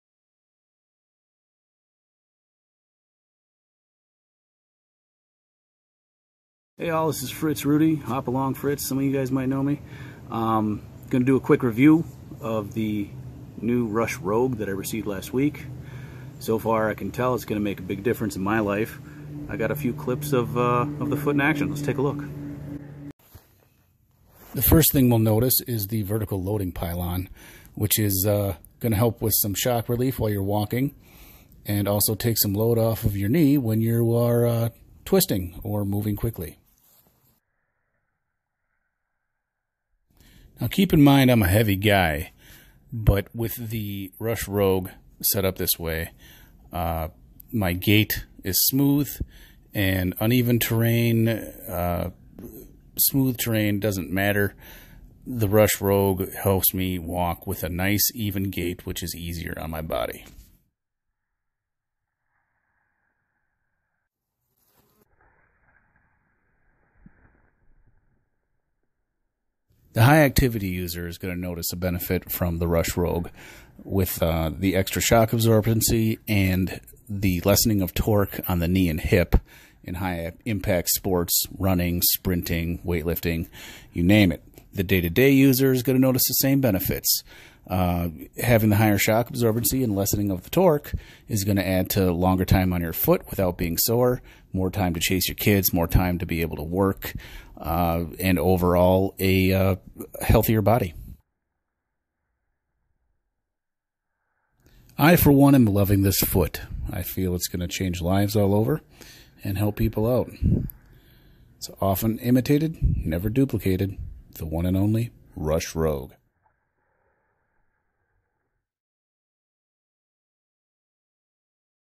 Fritz "HopAlong" Rudy Goes ROGUE
Longtime Rebel Fritz Rudy spent many years on his RUSH HiPro® before switching to the RUSH ROGUE®.  Watch as Fritz puts his ROGUE's Vertical Loading Pylon to work.